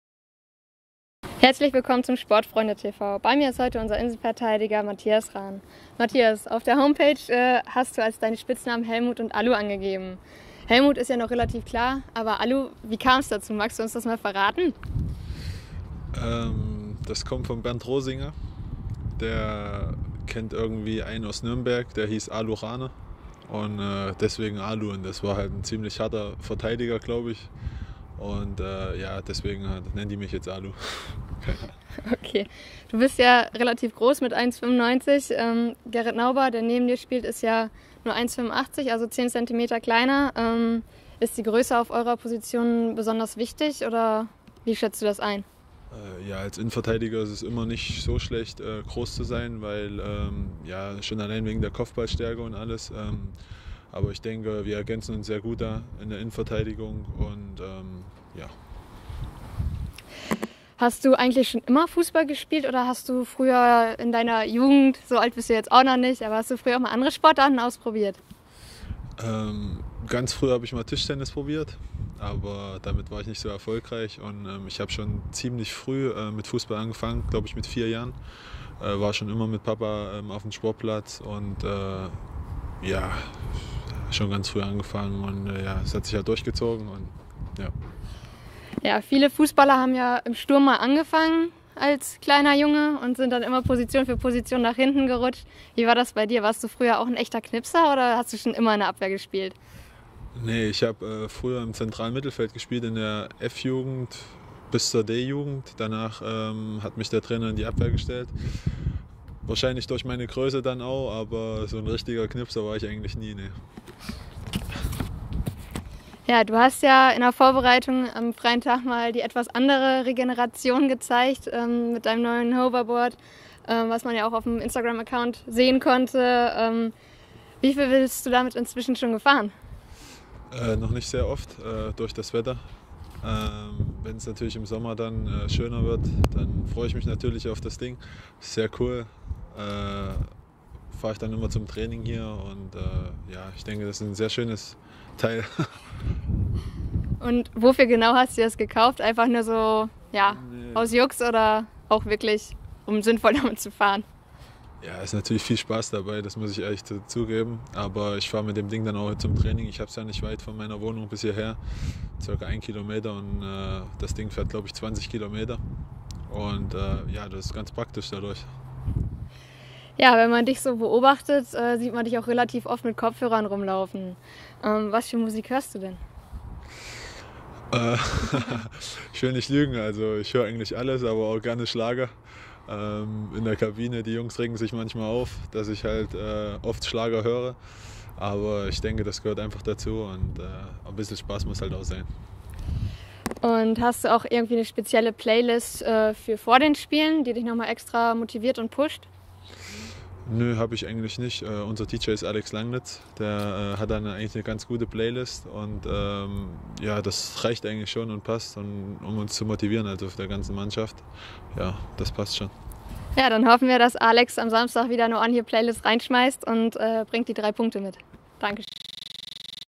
Interview mit Matthias Rahn
Matze Rahn wie ihr ihn noch nicht kennt. Von Spitznamen bis zu seinen Musikgeschmack.